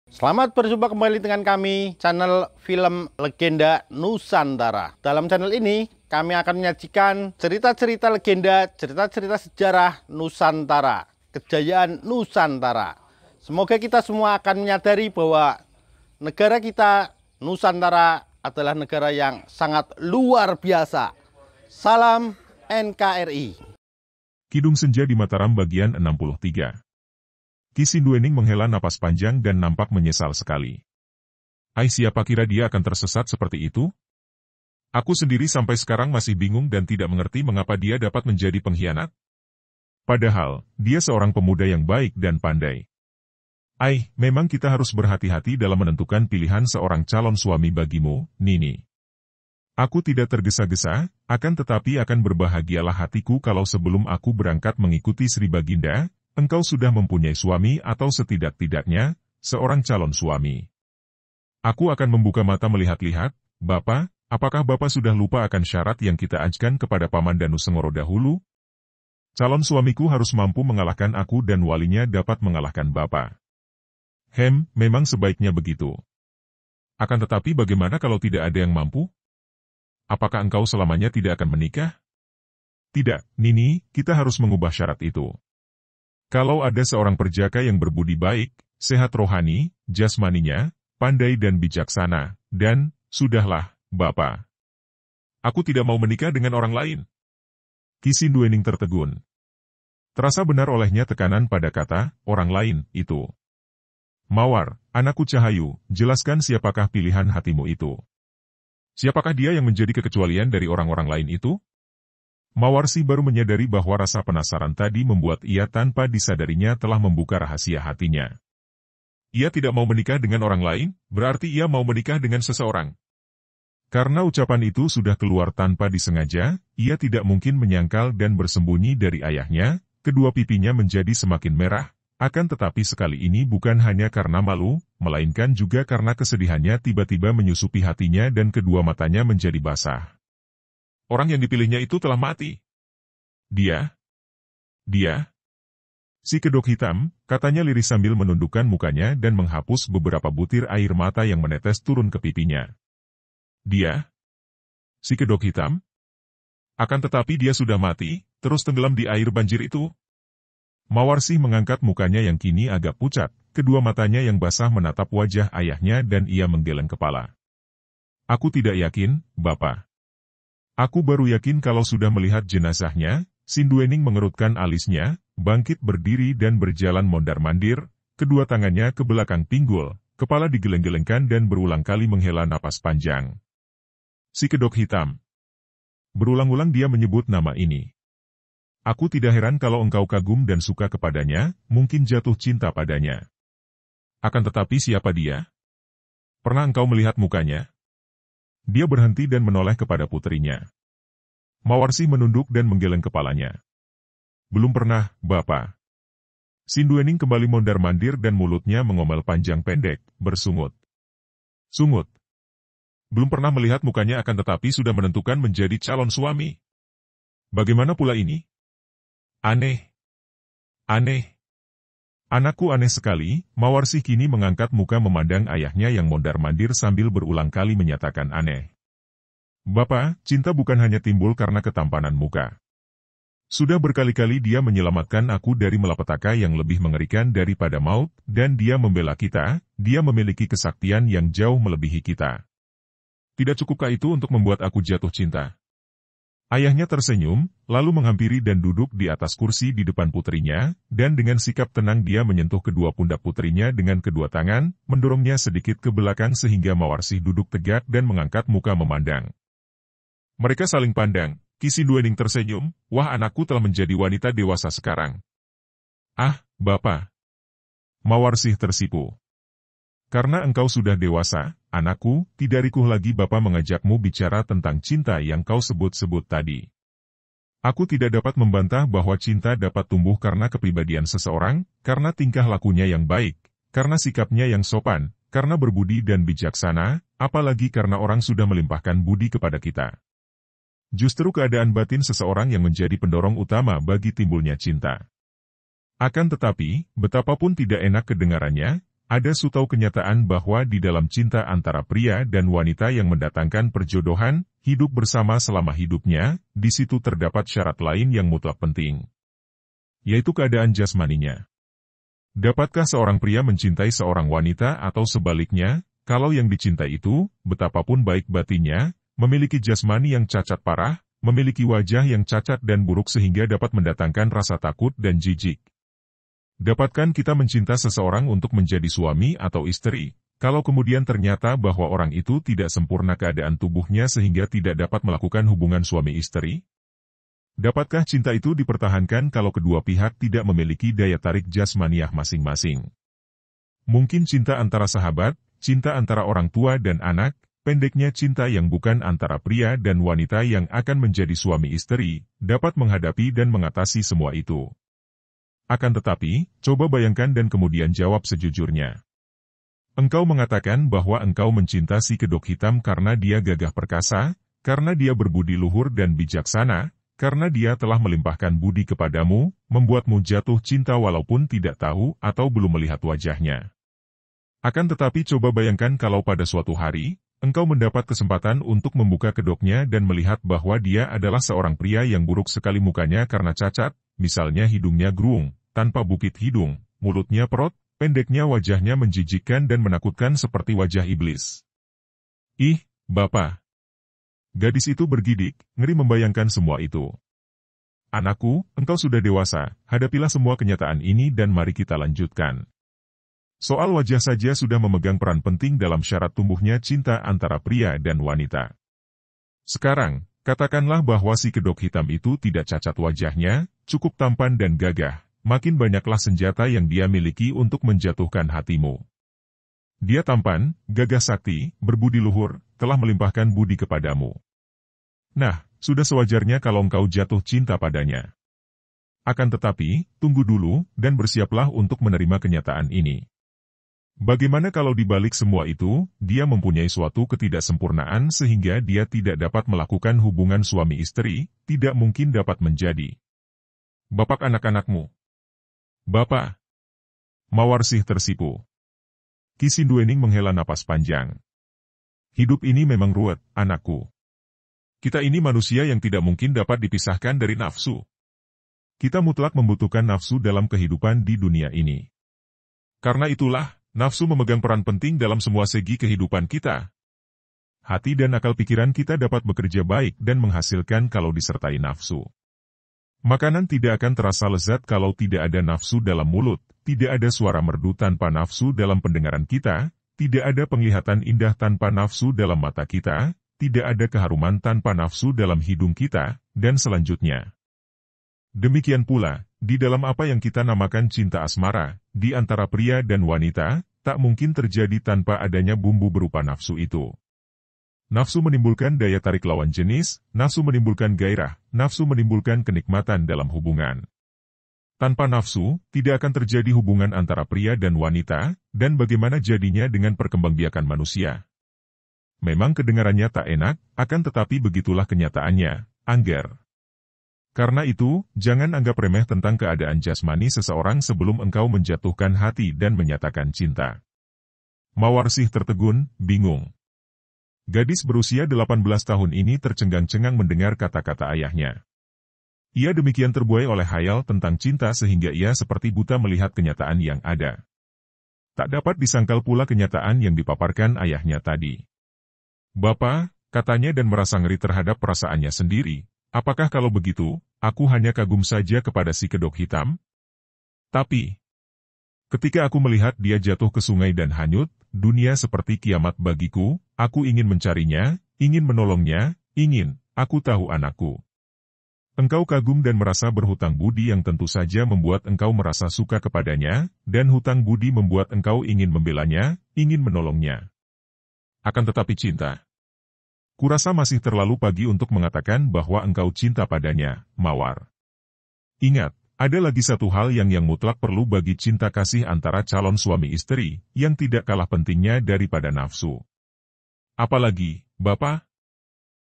Kidung Senja di Mataram Bagian 63
Ki Sinduwening menghela napas panjang dan nampak menyesal sekali. ”Aihhh siapa kira dia akan tersesat seperti itu? Aku sendiri sampai sekarang masih bingung dan tidak mengerti mengapa dia dapat menjadi pengkhianat! Padahal, dia seorang pemuda yang baik dan pandai. Aihhh, memang kita harus berhati-hati dalam menentukan pilihan seorang calon suami bagimu, nini. Aku tidak tergesa-gesa, akan tetapi akan berbahagialah hatiku kalau sebelum aku berangkat mengikuti Sribaginda, engkau sudah mempunyai suami atau setidak-tidaknya, seorang calon suami. Aku akan membuka mata melihat-lihat……” “Bapa,